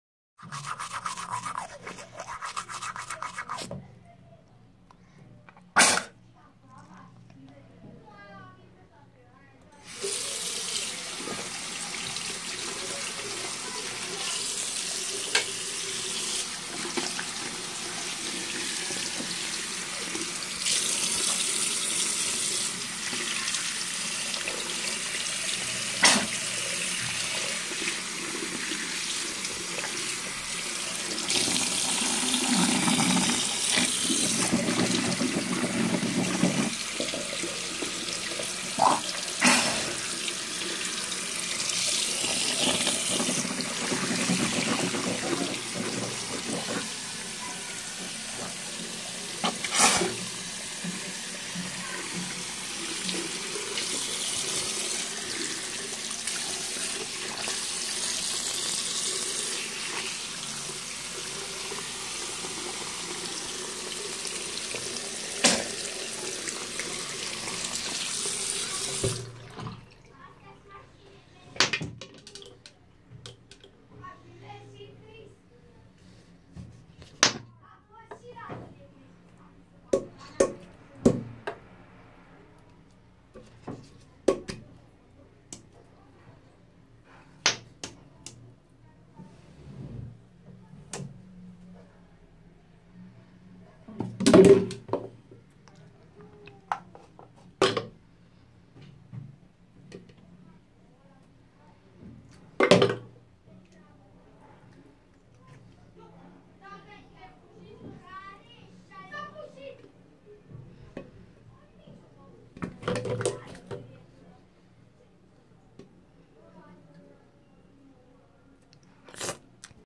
How To Brush Your Teeth
In this video i will show you how i brush my teeth. Kinda stupid right?